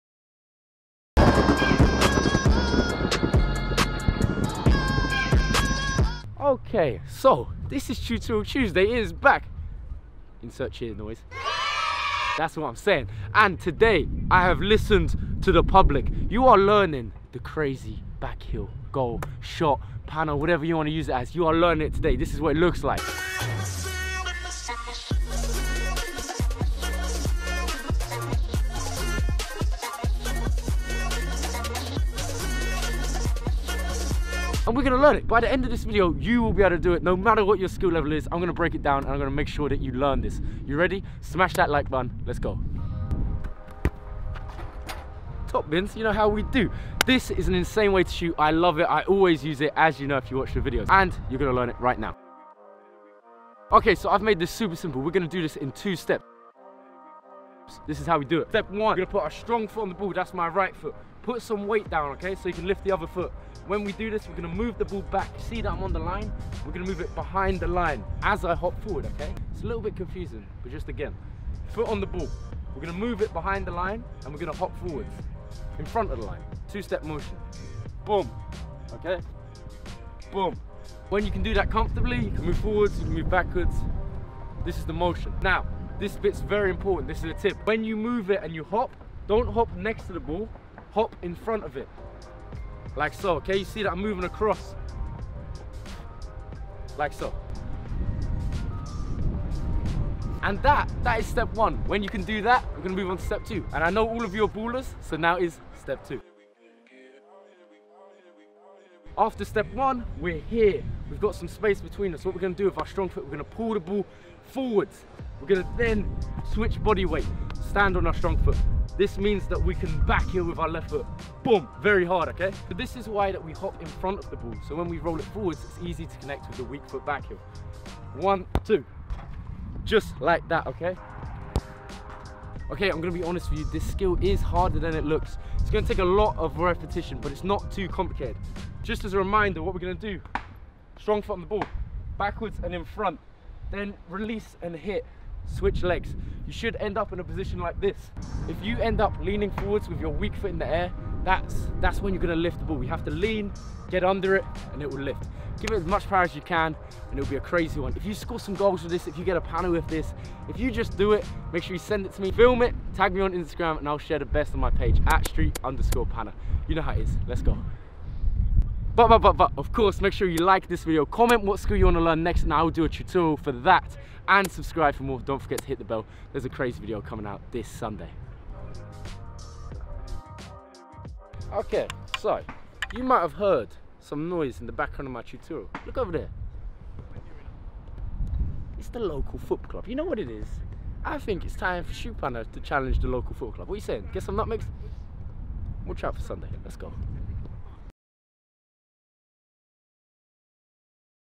Learn Insane Back Heel Trickshot!! Street Panna Tutorials!
Yes Yes! The Seventh TutorialTuesday is here!!
I Love using this Skill, it is super effective, so I look forward to seeing you use it and Scoring some banger with it!
Film yourself doing it and tag me in it on Instagram!

Y'all the MVPs